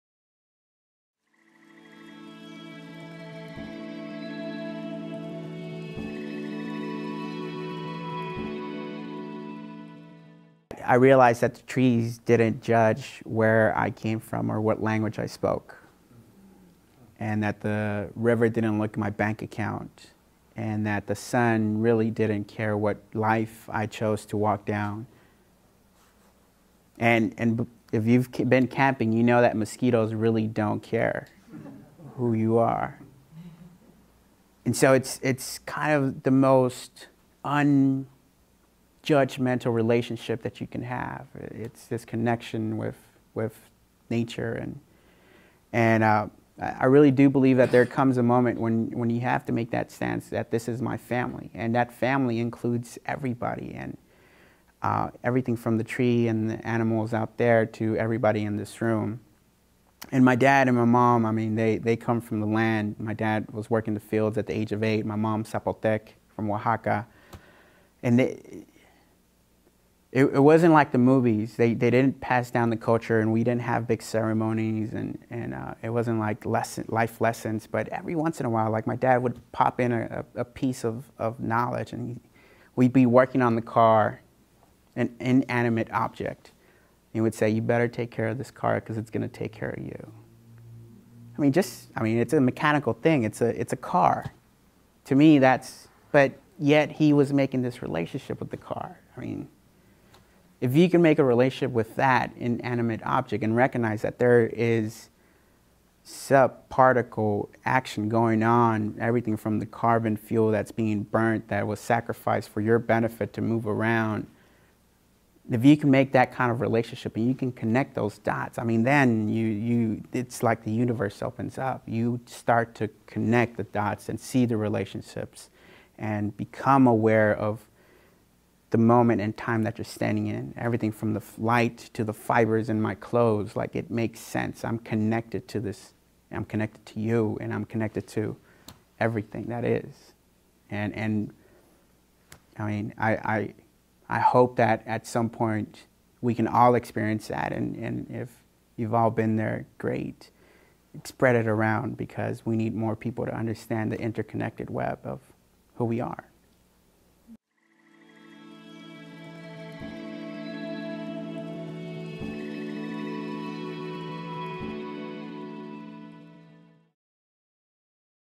Juan Martinez Video: Nature is Non-Judgemental
Manzanita Speaker Series presents Juan Martinez, "Exploring the Urban Wild". National Geographic Explorer, TED Speaker, and a proud product of south central Los Angeles, Juan D. Martinez leads us in a discussion about urban communities, technology, and nature.